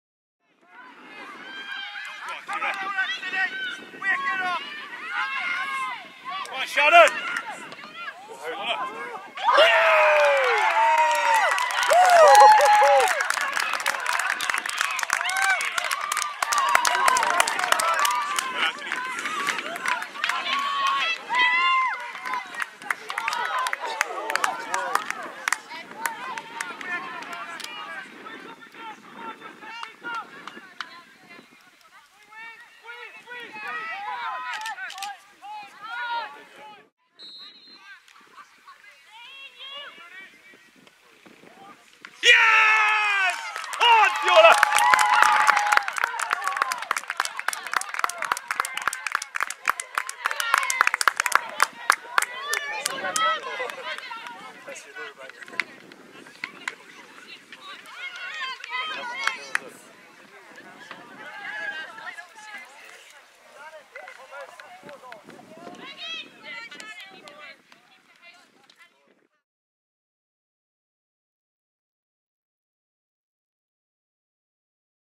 Fiona Ryan | Two stunning goals for the U19s
Fiona Ryan with two stunning goals in Wexford Youths Women's U19s 2-0 win over Cork City FC Women.
Mayfield United FC, Cork 18th July 2021